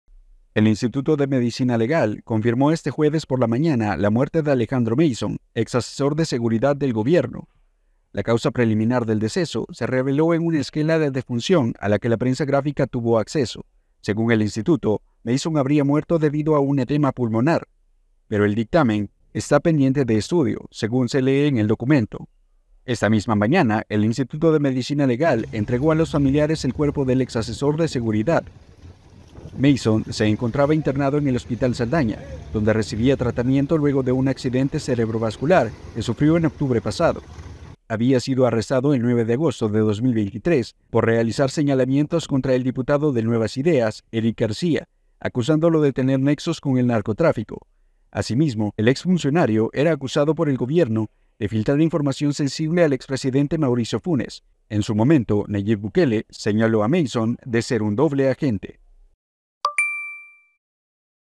La familia de Alejandro Muyshond confirma el deceso del exasesor de seguridad
El exasesor estaba ingresado en el Hospital Saldaña, donde recibía atención después de un accidente cerebrovascular.